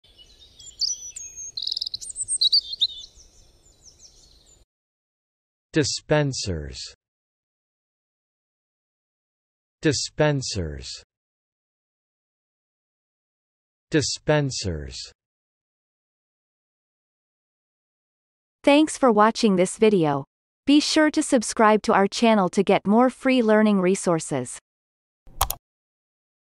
How to pronounce dispensers - Vocab Today
The pronunciation of the word dispensers in Amercian accent is demonstrated in this video. If the word is from another language, such as brand name, it will be pronounced in its original language. Based on the word frequency, this is of 4 stars difficulty.